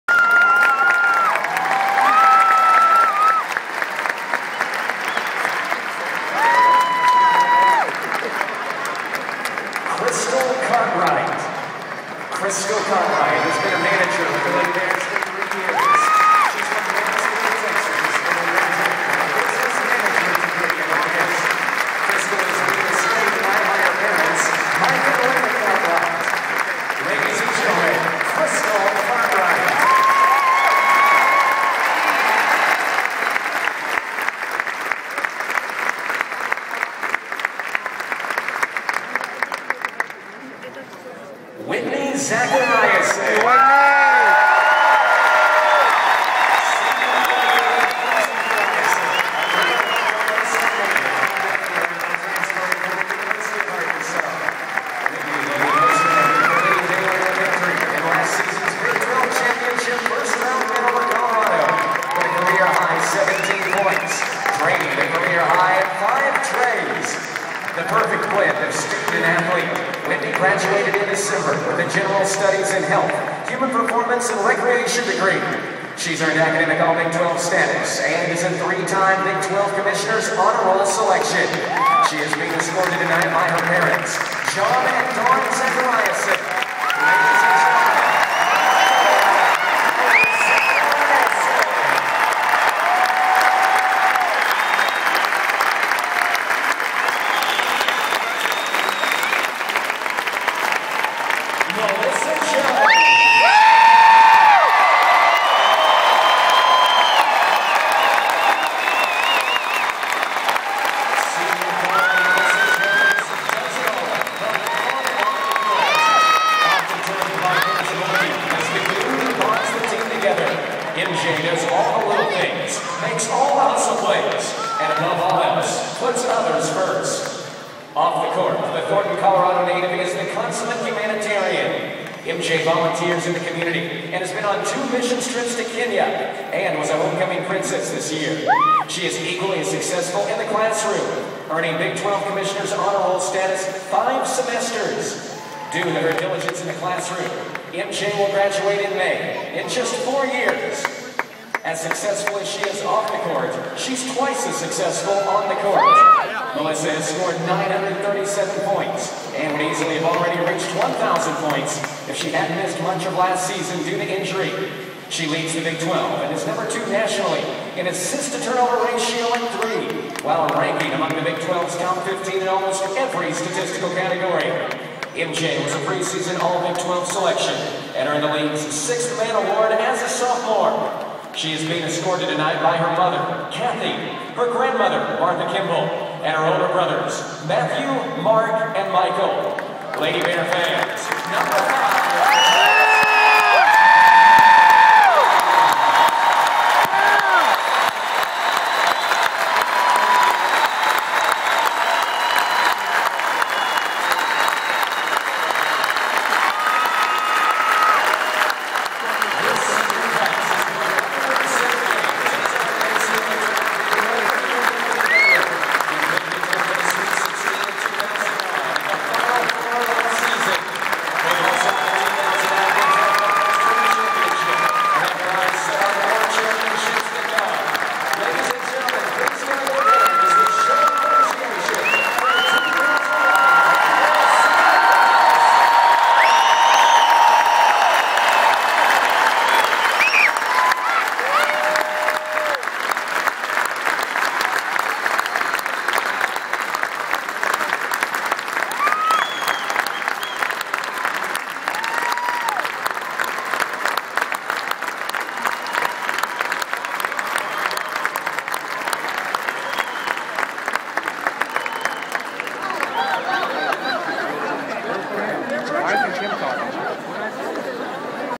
Baylor WBB Senior Night Ceremonies
The halftime ceremonies honoring the seniors on the Baylor Women's Basketball team - Melissa Jones, Whitney Zachariason.